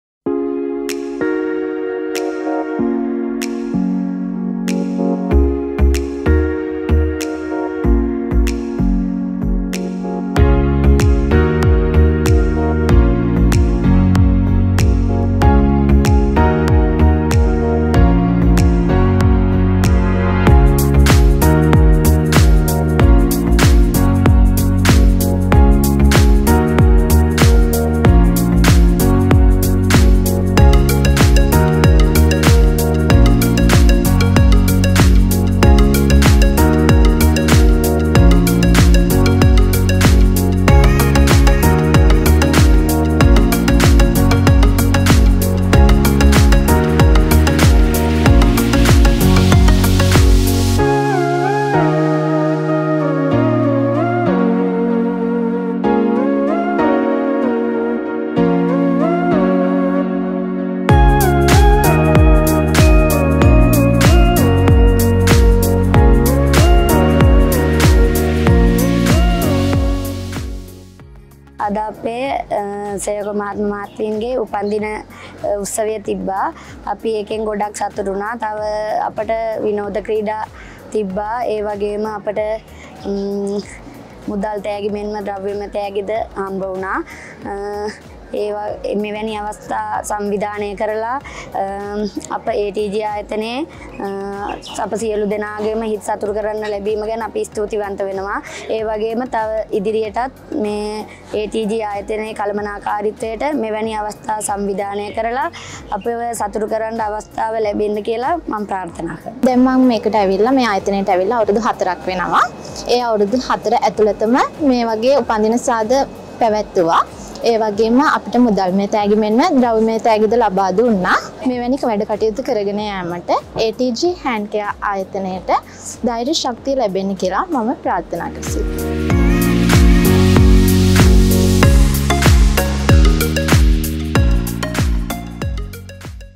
SERENITY OF ANCIENT SRI LANKA SWEEPS OVER BIRTHDAY! | ATG Hand Care PVT LTD - Katunayake
කටුනායක ATG හෑන්ඩ් කෙයාර් ආයතනයේ මෙවර මාසික උපන්දින සැමරුම සුන්දර ගැමි පරිසරයක් තේමාව කරගනිමින්, මැයි මස නව වන (09) දින සංවිධාන කර තිබිණි.
ගැමි පාරිසරික අත්දැකීම් වලින් සමන්විත වූ මෙම උත්සවයේ දී, මාර්තු දහසය (16) වන දා සිට අප්‍රේල් පහළොස් (15) වන දා දක්වා උපන්දින සැමරූ කටුනායක ATG හෑන්ඩ් කෙයාර් ආයතනය නියෝජනය කළ සේවක මහත්ම මහත්මීන් පනස් එක් (51) දෙනෙකු වෙත වටිනා උපන්දින ත්‍යාග පිරිනැමීම සිදුවිය.
A birthday celebration encapsulating the beauty and tranquility of an ancient rural Sri Lankan village was held at ATG Hand Care PVT LTD in Katunayake on the ninth (09) of May.
Comprising experiences in Sri Lanka's rural environments, decorations were set up at the premises ahead of the celebrations. A total of fifty-one (51) employees, who celebrated their birthdays from March sixteenth (16th) to April fifteenth (15th), received special gifts at the event. A fun game was also held at the event.
ATG‍ ආයතනය මගින් ඔබගේ රසාස්වාදය හා වින්දනය සදහා සංවිධානය කරන මෙවැනි වැඩසටහන් නැරඹීමට අප සමග රැදී සිටින්න.
Stay with us to watch events organized by the ATG Group for your entertainment.
මෙම වීඩියෝව ඔබගේ මිතුරන් අතරේ බෙදා හරින්න අමතක කරන්න එපා! ඒ වගේම මේ අත්දැකීම ගැන ඔබගේ අදහස් අපිට කියන්න.